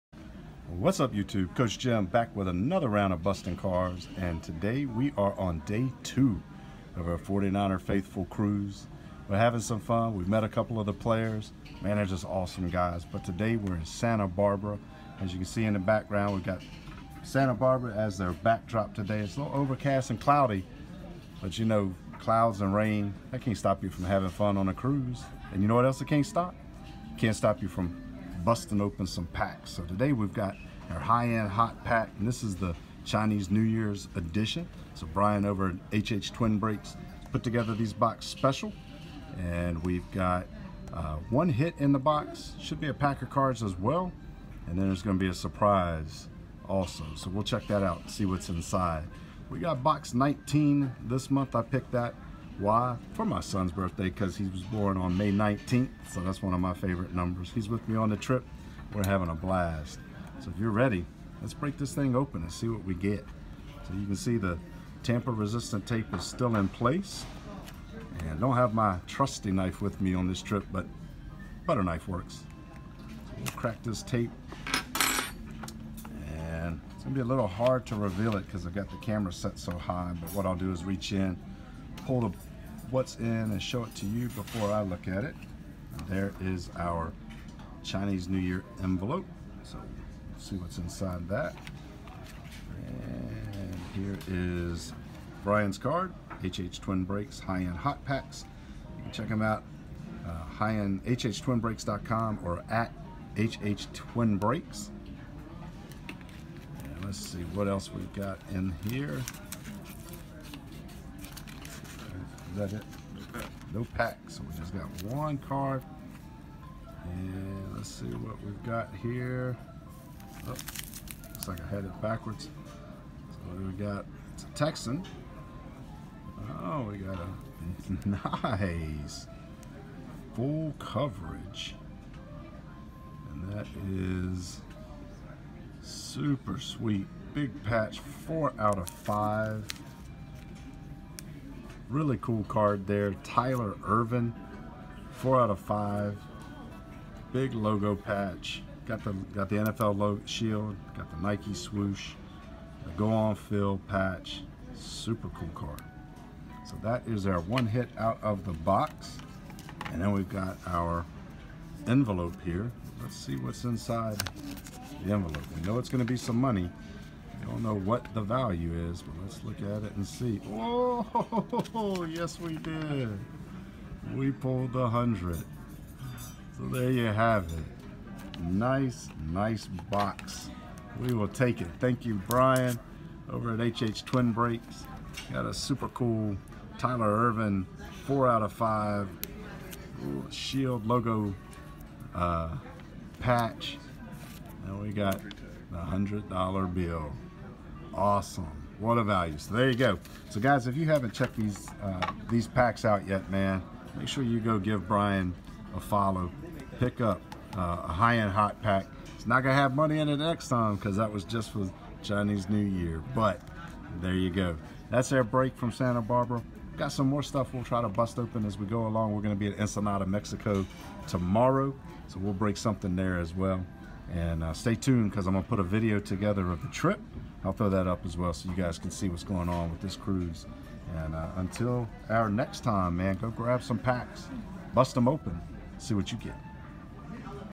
High End Hot Pack Chinese New Year Edition! Most Unique 1/1 Ever!
Shot this High End Hot Pack Chinese New Year Edition video on the cruise but due to crappy Internet had to wait to upload it till we got home. Got a special HHTwinBreak 1/1 in this one! Check it out!